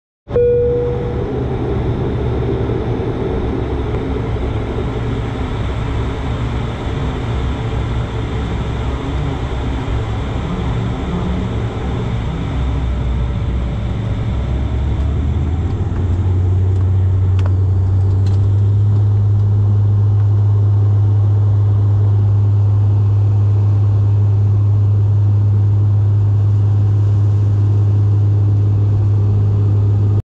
Air Canada Bombardier Q400 acceleration at Seattle-Tacoma International Airport(SEA) [Not a takeoff]
Air Canada Bombardier Q400 acceleration at Seattle-Tacoma International Airport(SEA) which is going to Vancouver International Airport(YVR). I do apologize because this was supposed to be a takeoff video but my gorpo was low on power and died.